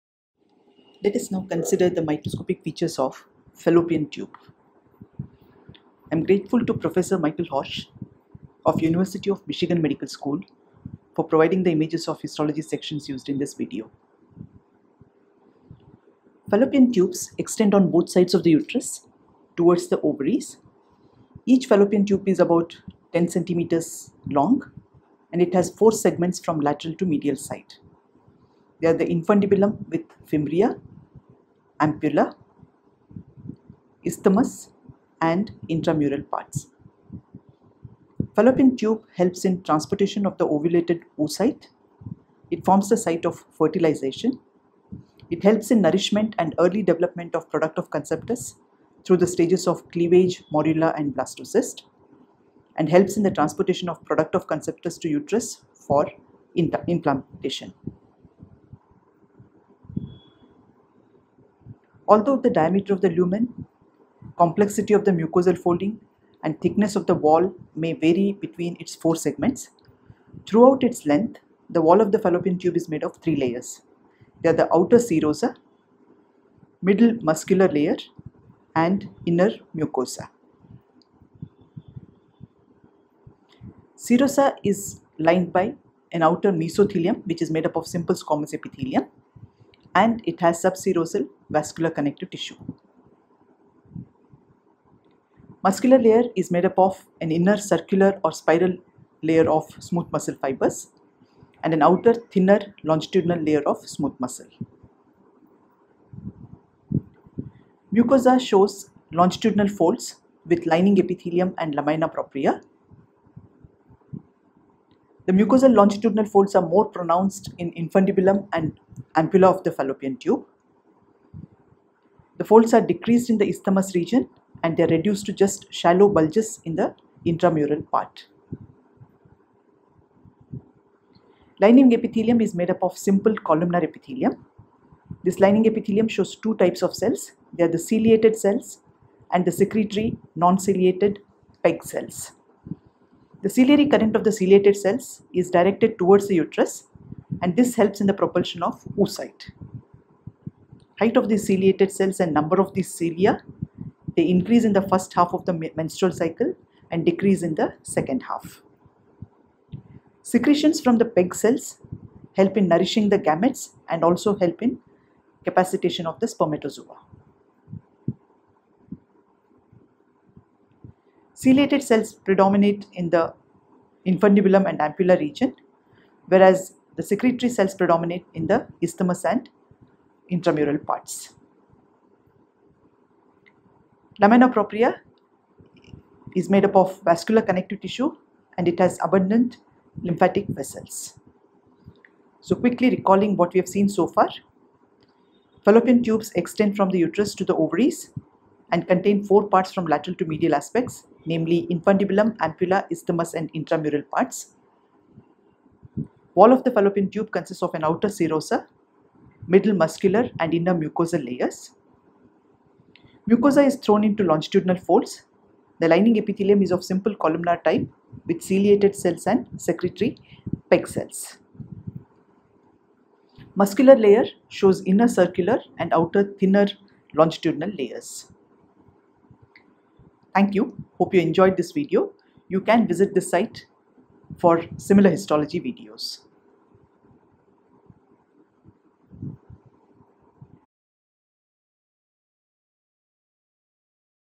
Histology with Chimmalgi: Fallopian Tube
Microscopic features of fallopian tube